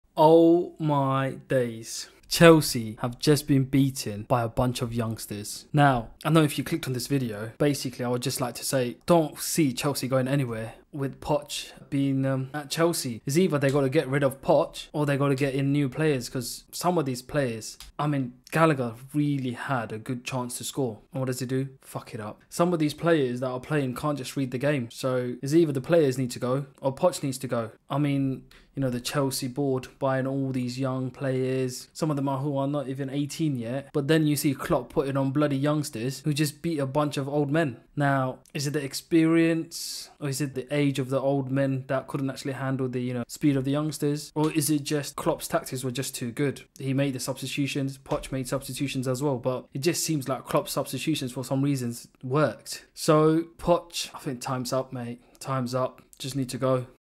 Unbiased fan reaction to Chelsea v Liverpool Carabao Cup Final
unbiased fan reaction video for the thrilling EFL Cup clash between Chelsea and Liverpool! Watch as passionate fans share their raw emotions and insights on the game that saw Liverpool's young talents shine against Chelsea. From stunning goals to nail-biting moments, we cover it all with unbiased analysis and genuine reactions. Whether you're a die-hard Chelsea supporter or a loyal follower of Liverpool, this is the place to be for post-match discussion.